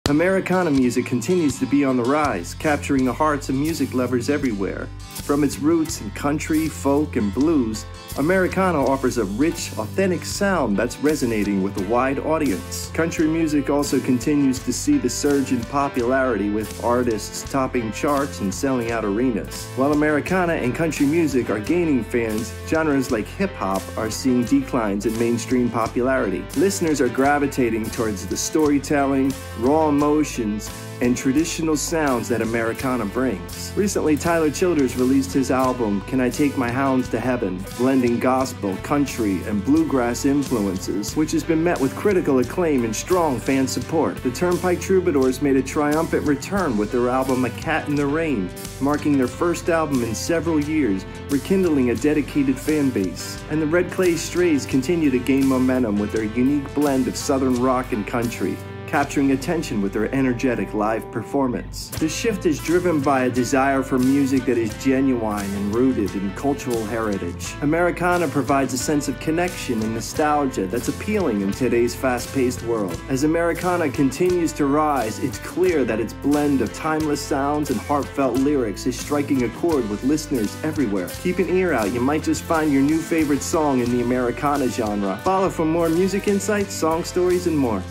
"Americana's Rise: The New Wave of Authentic Sound"
Americana music is capturing the hearts of fans everywhere, rising in popularity alongside country music while genres like hip hop see a slight decline. Artists like Tyler Childers, The Turnpike Troubadours, and The Red Clay Strays are leading the charge with new releases and energetic performances.

Americana's authentic sound, rooted in folk, country, and blues, offers a connection and nostalgia that's resonating in today's fast-paced world. Follow for more music insights and trends!